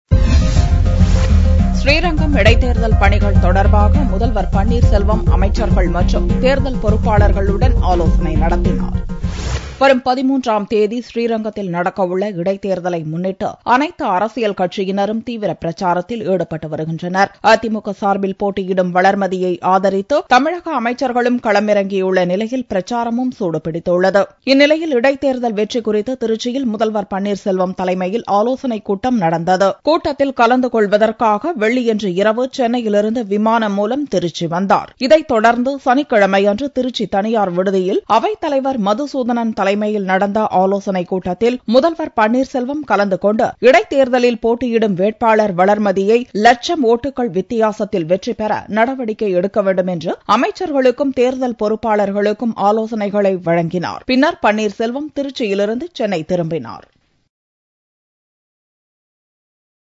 Tamilnadu Chief Minister O. Panneerselvam in Discussion on Election at Trichy
Tamilnadu Chief Minister O. Panneerselvam in Discussion on Election at Trichy - Dinamalar Feb 7th 2015 Tamil Video News For more videos